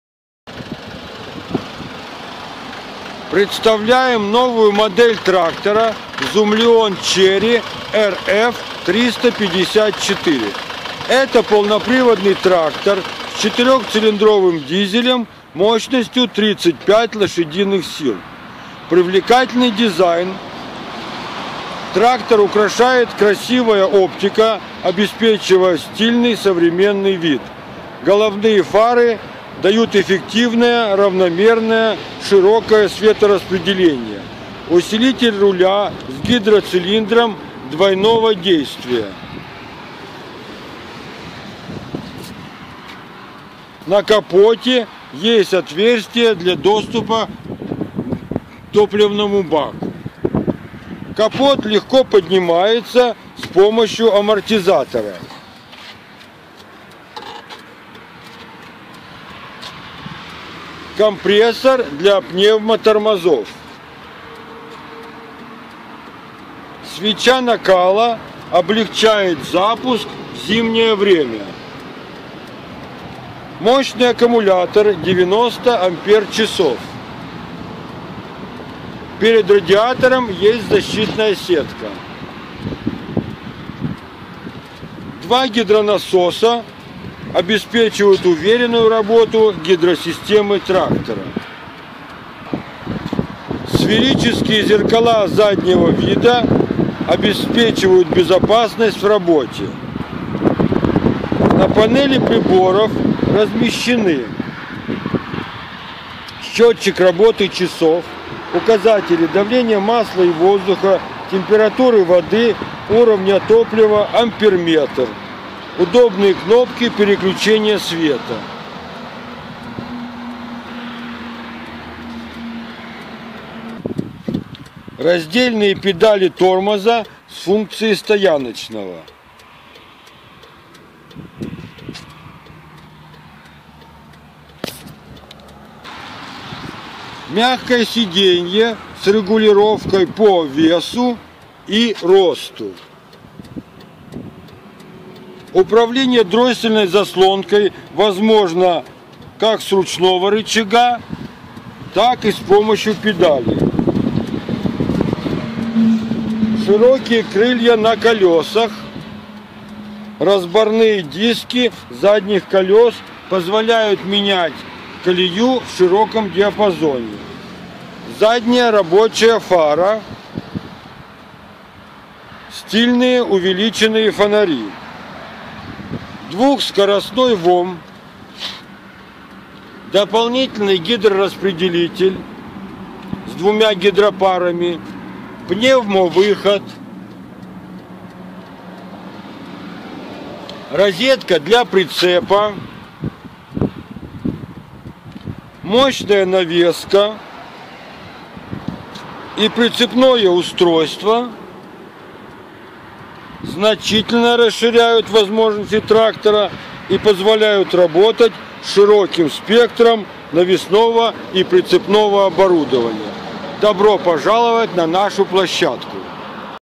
Минитрактор Zoomlion RF 354B
Минитрактор Zoomlion RF 354B обладает рядом преимуществ перед аналогичными моделями других брендов. Основным преимуществом минитрактора является эксклюзивный дизельный двигатель, разработанный австрийскими и английскими специалистами с упором на современность, экономичность и надёжность. Подробнее